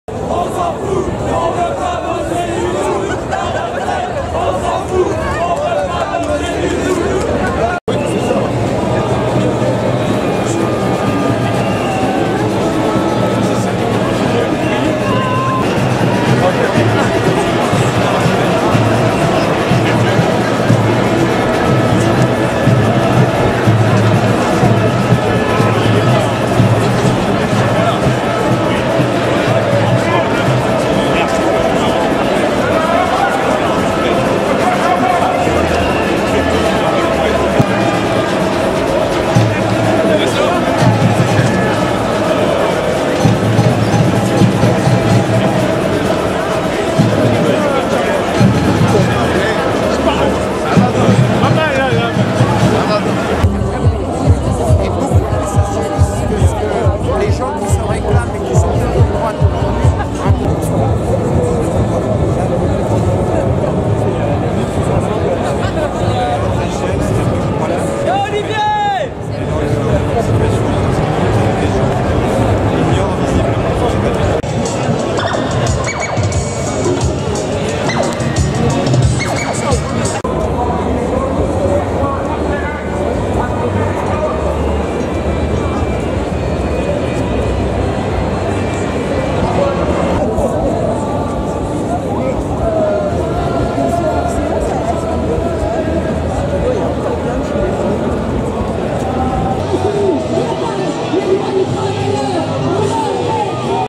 Jean-Luc Mélenchon, Olivier Faure, Philippe Martinez etc @ Manifestation Retraites le 7 février 2023
Jean-Luc Mélenchon, Olivier Faure, Philippe Martinez etc @ Manifestation contre la loi Retraites le 7 février 2023